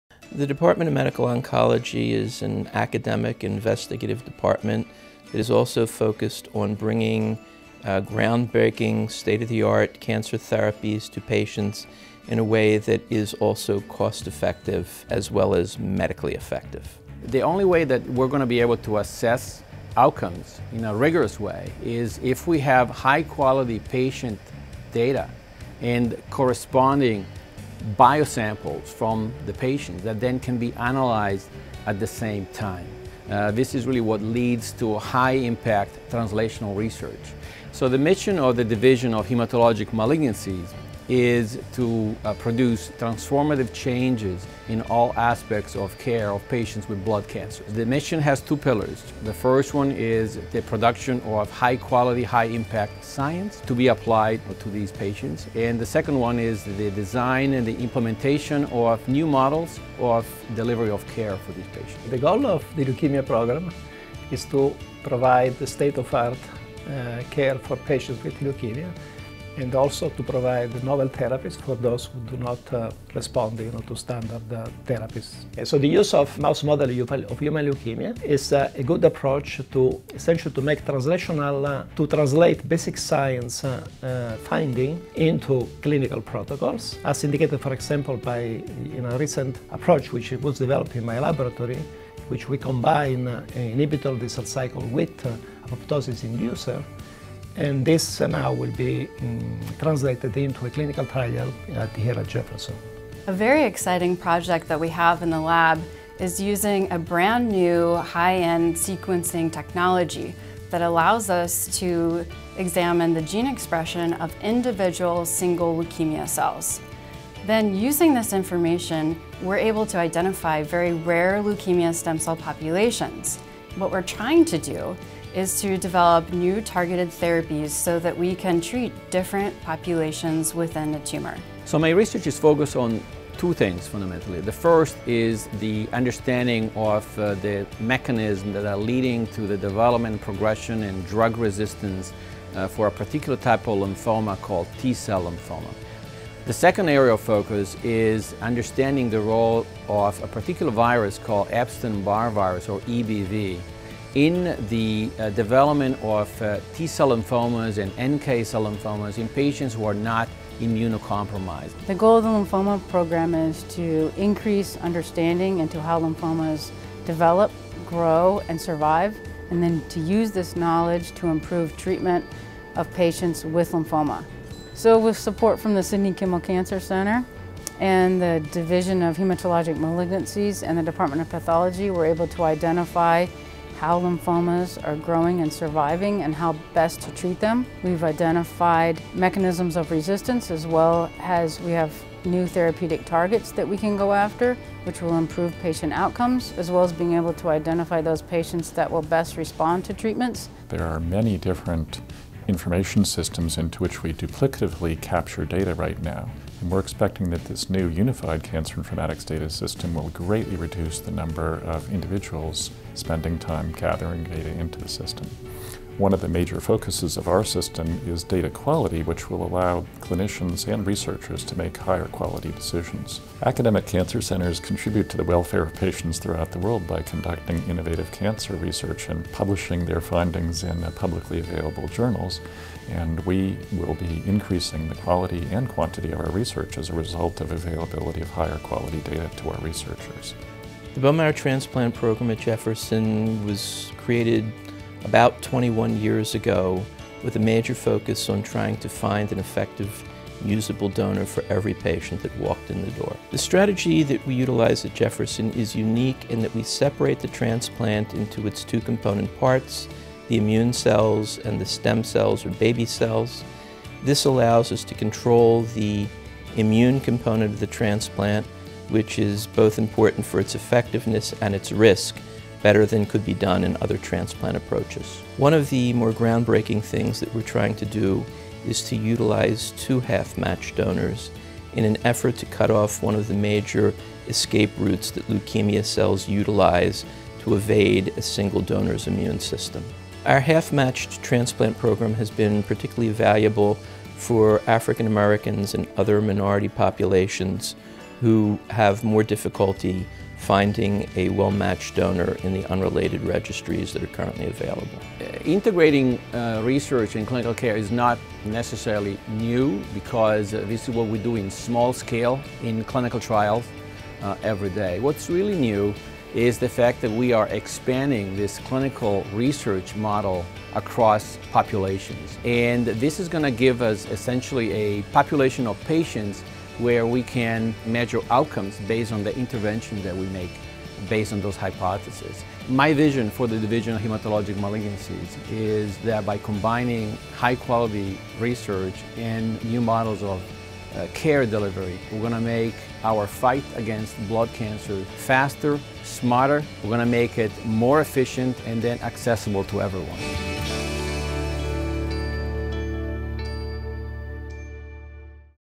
Sidney Kimmel Cancer Center at Jefferson: Innovative Cancer Care Delivery
The Division of Hematologic Malignancies and Hematopoietic Stem Cell Transplantation of the SKCC is the place where transformative advances in our knowledge of all aspects of blood cancers (causes, risk, diagnosis, prognosis, treatment, and survivorship) are taking place and new models in the delivery of personalized cancer care are being designed and implemented.